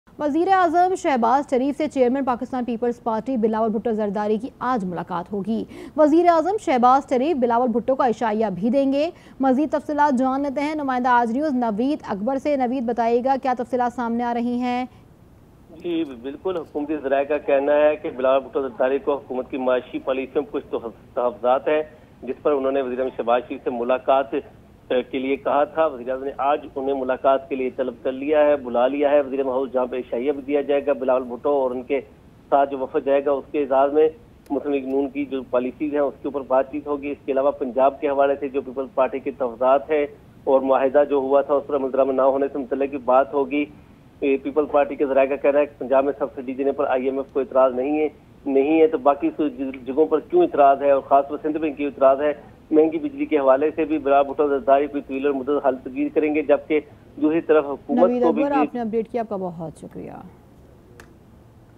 PM Shehbaz Sharif invitation to Bilawal Bhutto for dinner, discussion on important issues - Aaj News
PM Shehbaz Sharif invitation to Bilawal Bhutto for dinner, discussion on important issues - 22 August 2024 - Aaj News

Spot Light With Munizae Jahangir:
shorturl.at/gwFU6